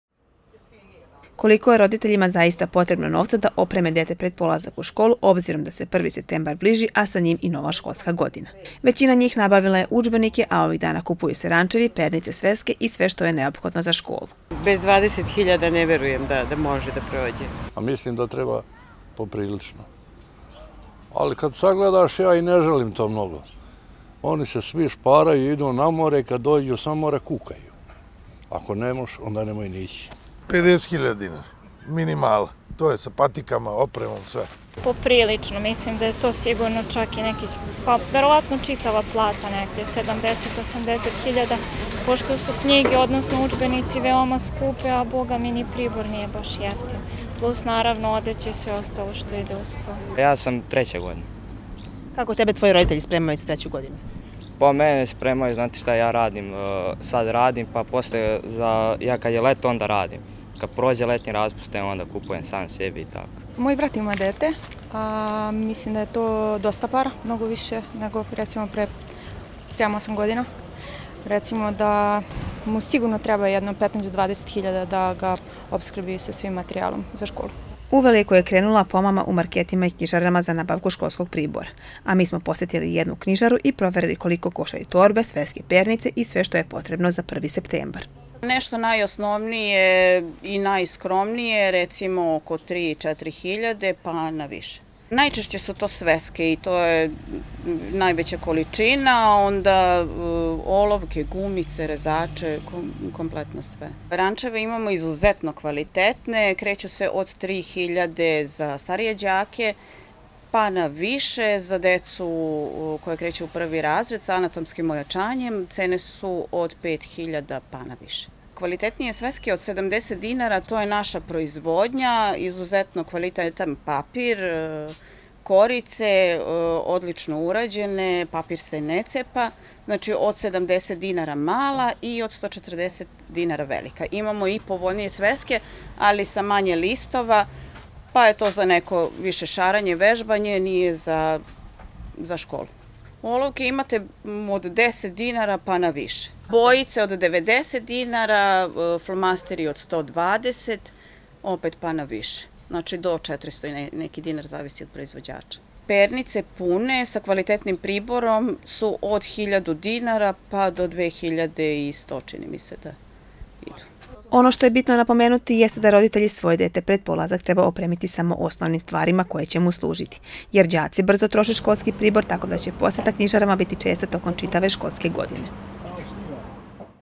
Koliko košta oprema đaka za školu?
Školski letnji raspust uskoro se završava, a deca i roditelji školaraca su tenutno zauzeti obilaskom knjižara i pravljenjem spiskova šta je to sve potrebno od pribora za početak nove školske godine, kao i koliko novca treba izdvojiti. Cene školskog pribora su raznolike, a osnovni pribor u knjižari može se pronaći i za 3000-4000 dinara.